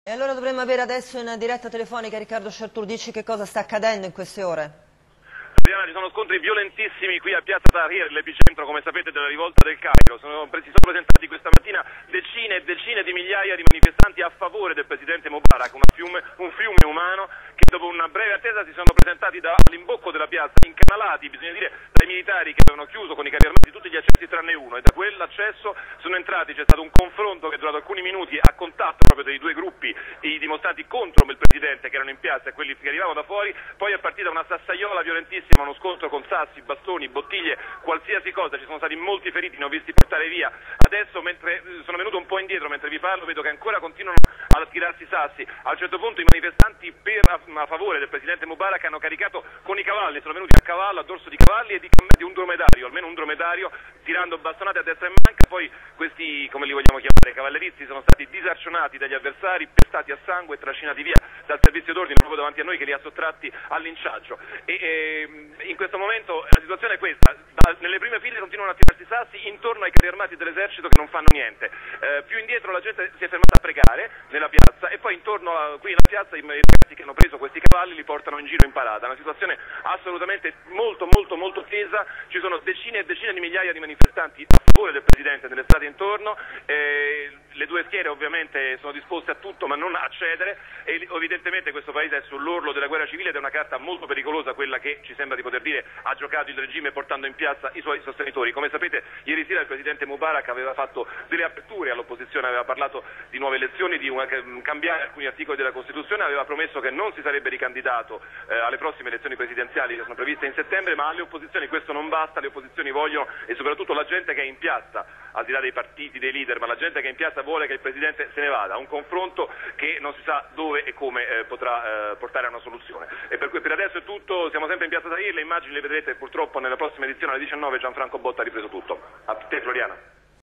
Scontri in Egitto pro e contro Mubarak
Scontri al Cairo tra manifestanti e sostenitori di Mubarak. Ignorati gli appelli delle forze armate. Obama: "La transizione deve cominciare ora". Servizio di Riccardo Chartroux e Gianfranco Botta